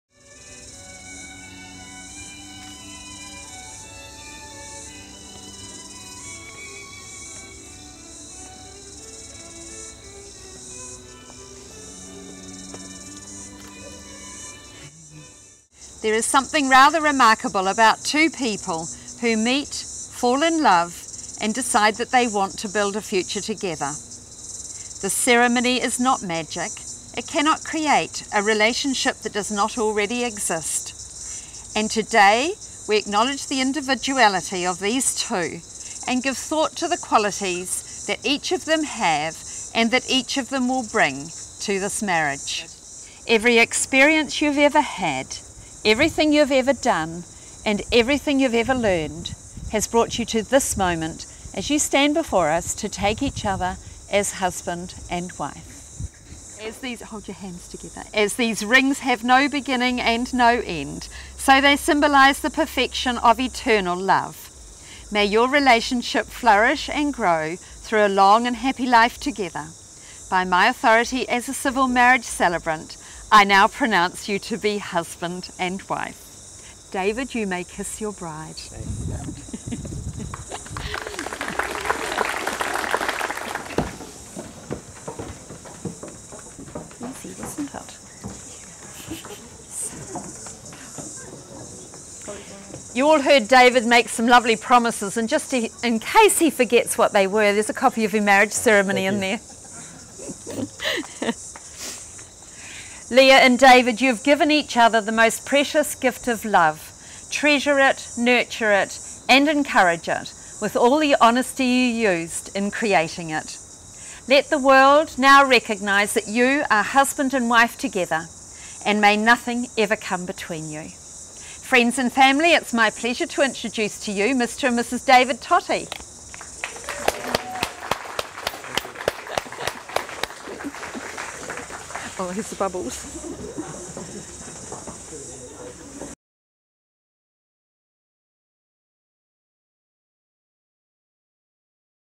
Kathy Bigwood, Registered Marriage Celebrant in Auckland
An introduction to Auckland based Kathy Bigwood registered marriage celebrant, here officiating at Alberton in Auckland.